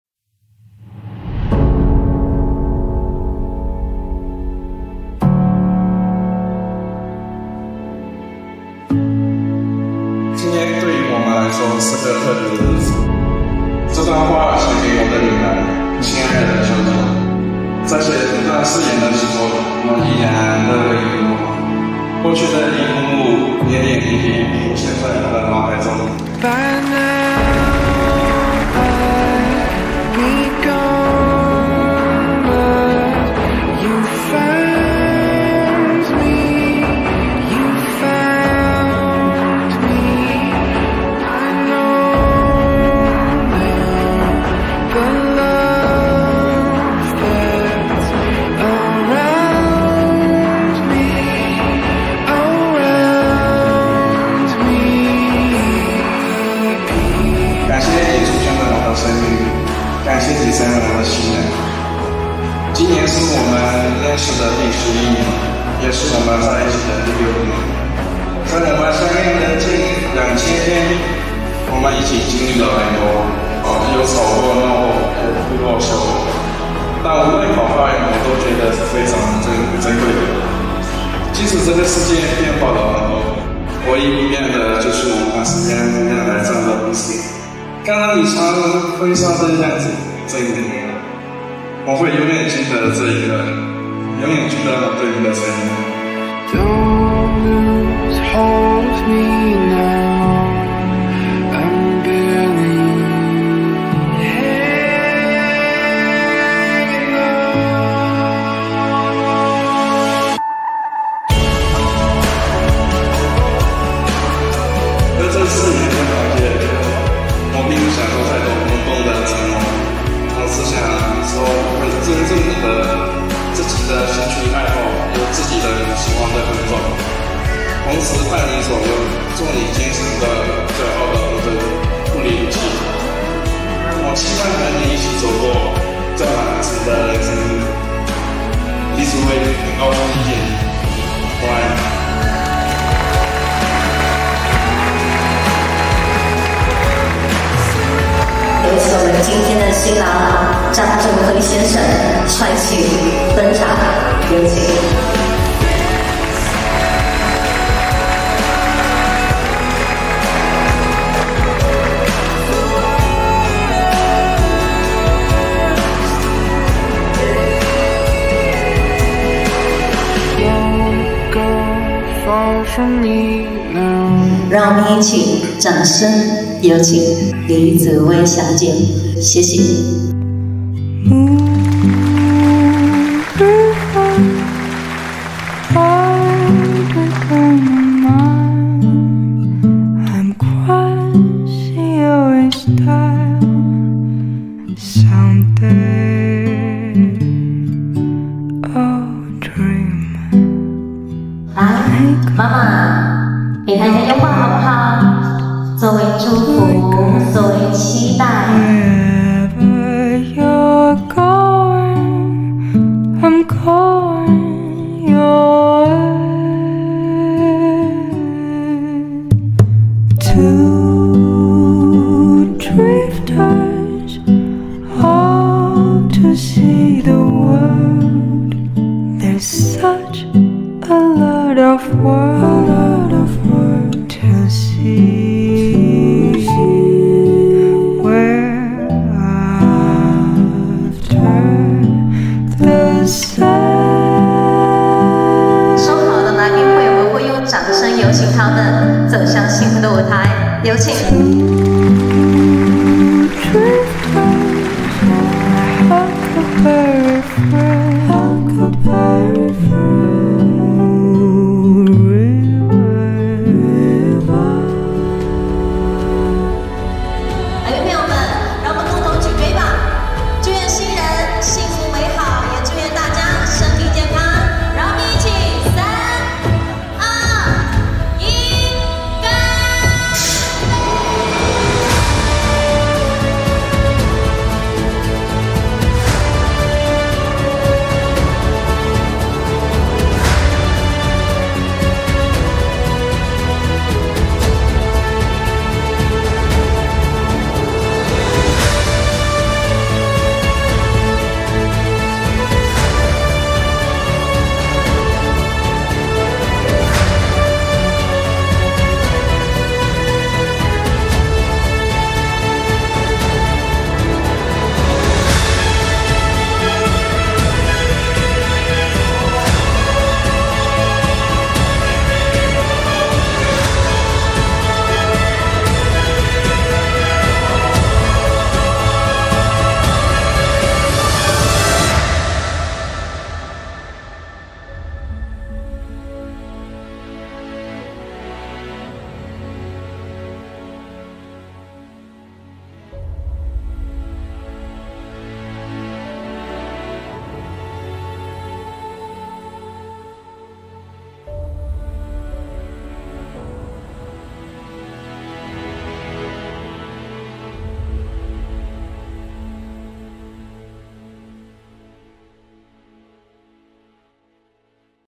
A romantic painted Wedding💕 at Orchid Grand Ballroom,Rupar Mandalar Resort
Dancing through life hand in hand,this enchanting couple paints our ballroom with the colors of their love.💞  Congratulations to Kyaw & Yoon!

Planning-Cattleya Wedding
Decoration-Cattleya Wedding
Venue-Orchid Grand Ballroom
Photo & Video - Moment Production